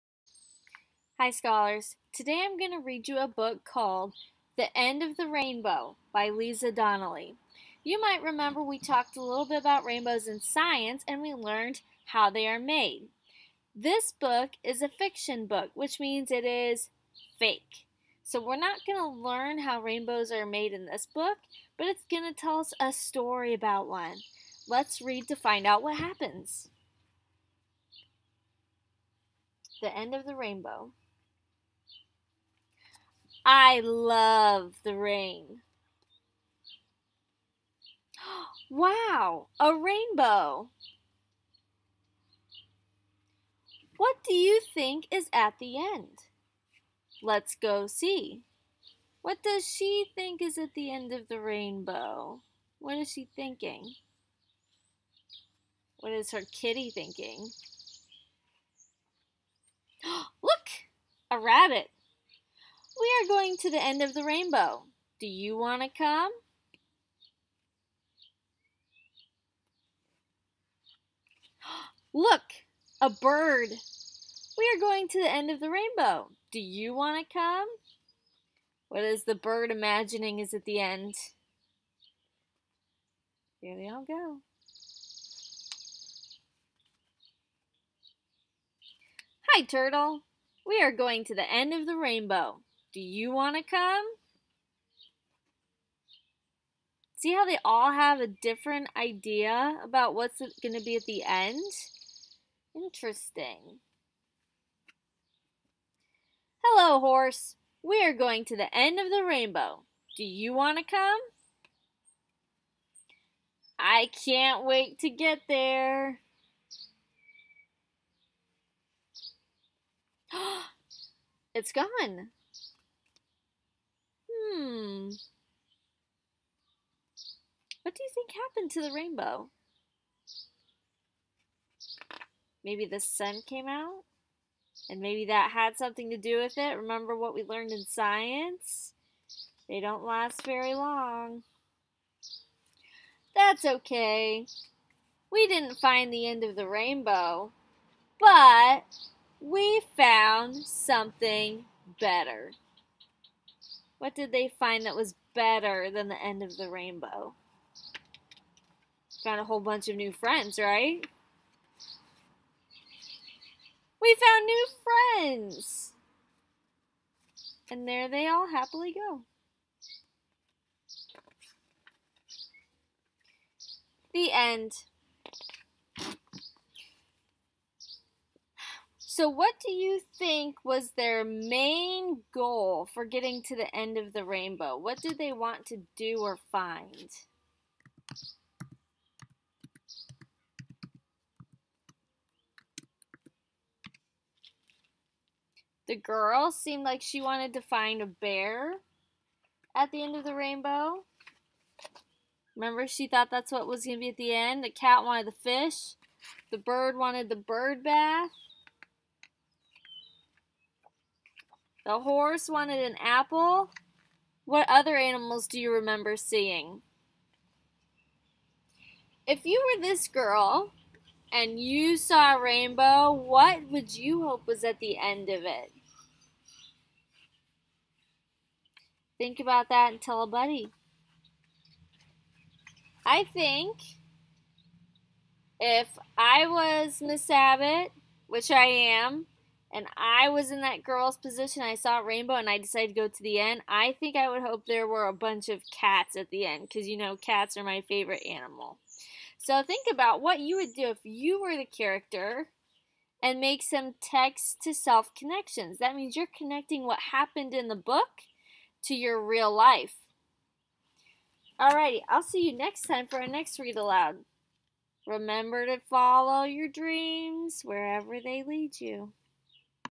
Miss Abbott Reads: The End of the Rainbow
Title: The End of The Rainbow
Author: Liza Donnelly

Watch the read-aloud and think about these questions:
1. Why did the characters want to find the end of the rainbow?
2. What was different about each animal's wish?
3. At the end of the story, the rainbow disappeared. What did the characters find instead?

Writing Extension: Draw a picture of a rainbow and show what you would want to find at the end of it. Then, write a story about your journey to find it! Remember to include complete sentences that start with a capital letter and end with a period.

Example: One day, after a scary storm, I saw a big rainbow in the sky! "I want to see what is at the end of it," I said. So I walked and walked for hours. Finally, I got to the end and saw a giant pot of cats there! We played until the rainbow disappeared. Now, I am not so scared of storms.